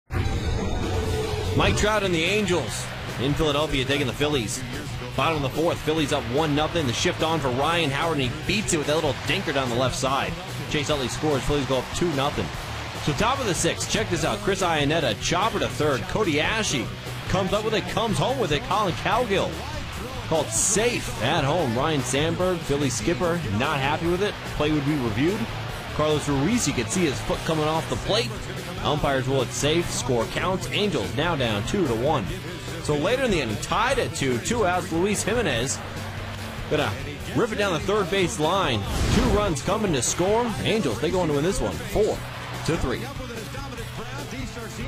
Jimenez, Angels Edge Phillies Los Angeles Angels vs Philadelphia Phillies
Luis Jimenez's two-run double in the sixth was the difference in the Angels' 4-3 win over the Phillies.